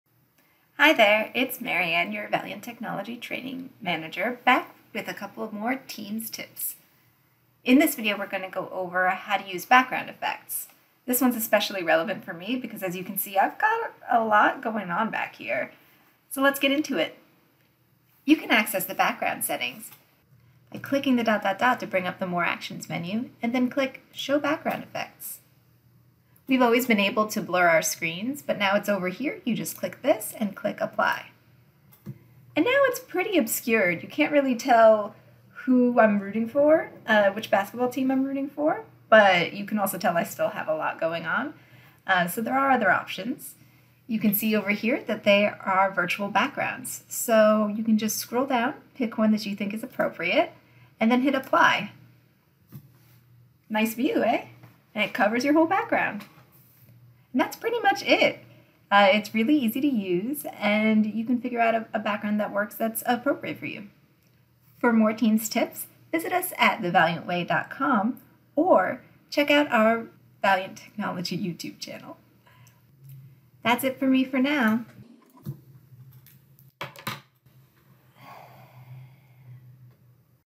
How to Apply Background Effects in Microsoft Teams
Learn how to use Teams Background effects including blur my background and virtual backgrounds. It's background-breaking technology!